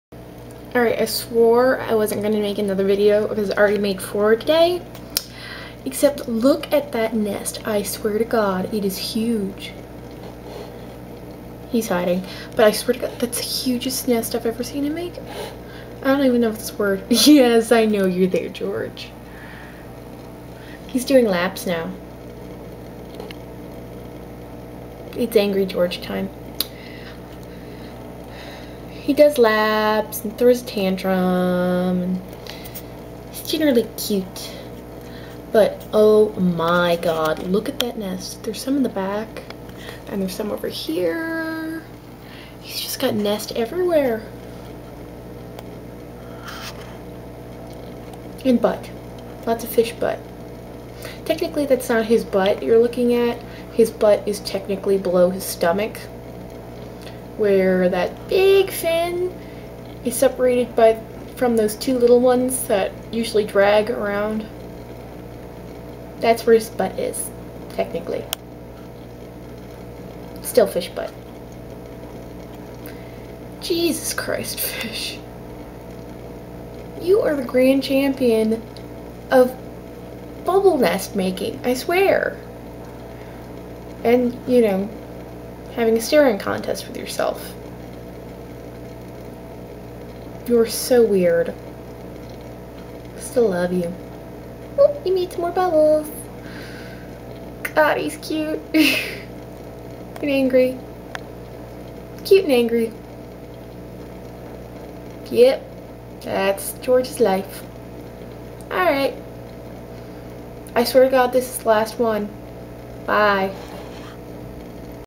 It Just Gets Bigger! (AKA George's Nest)
Y'know, I already said four videos in thirty minutes was enough.  Apparently not for George.  The nest GREW!  UPDATE (12/9/09): George has passed on to the Big Fish Tank.  He likely died overnight, and was found this morning, on the bottom of the tank.  He was very loved.